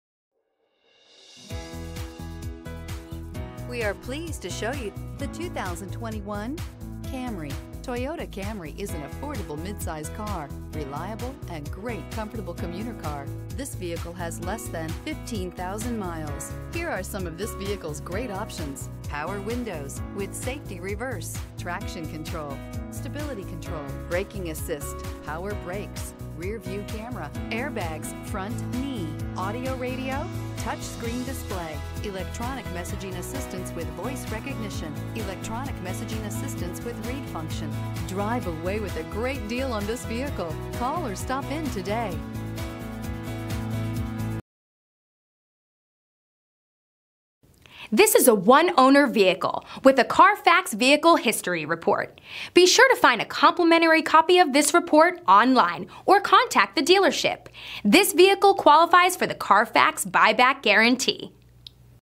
2021 Toyota Camry P8918
2021 Toyota Camry LE

AWD, Black Cloth. CARFAX One-Owner. Clean CARFAX. Blueprint 2021 Toyota Camry LE AWD 8-Speed Automatic 2.5L I4 DOHC 16V Recent Arrival! Odometer is 12946 miles below market average! 25/34 City/Highway MPG Priced below KBB Fair Purchase Price!